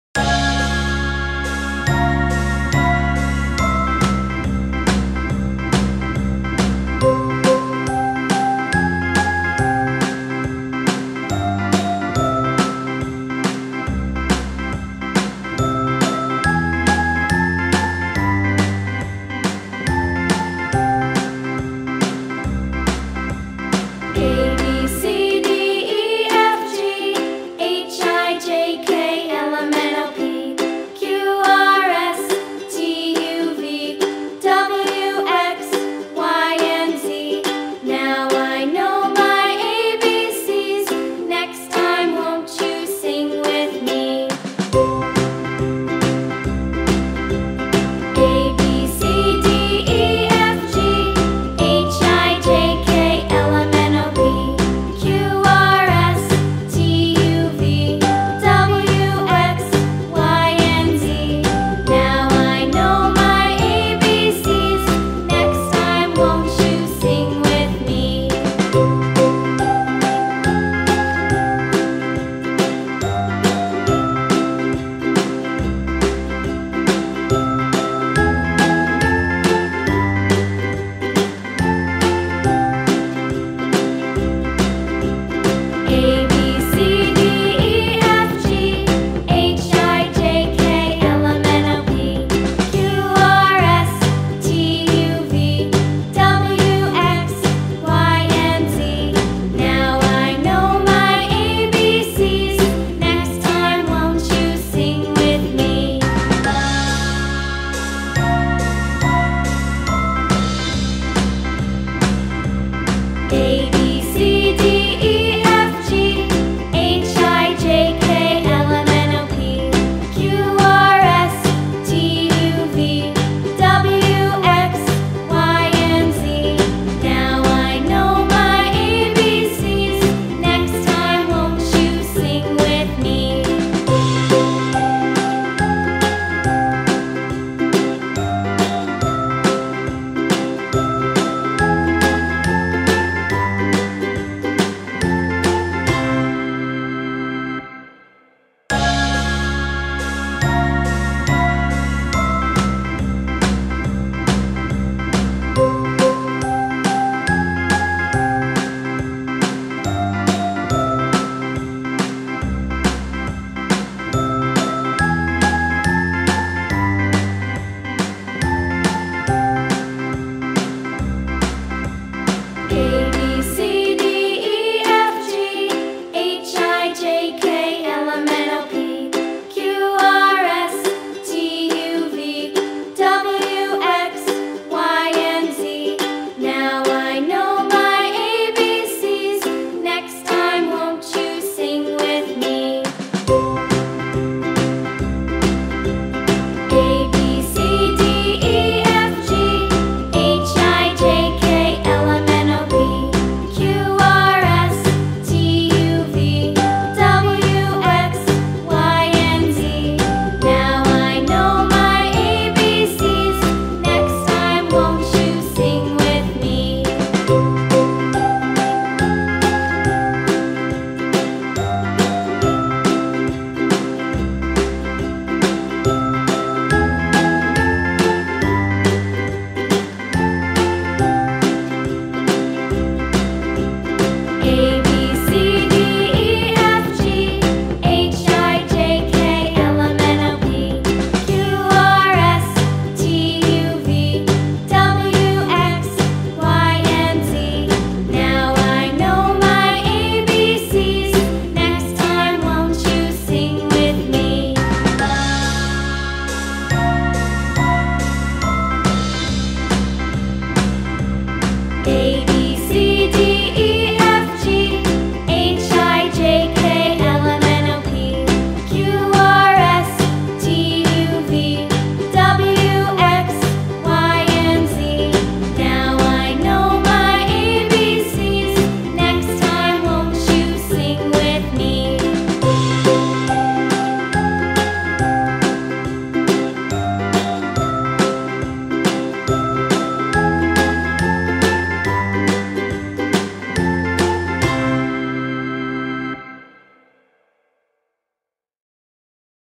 Easy Bride and Groom Drawing for Kids | Cute Painting and Coloring Tutorial for kids & toddlers
In this video, we'll show you step-by-step how to draw a cute Bride and Groom.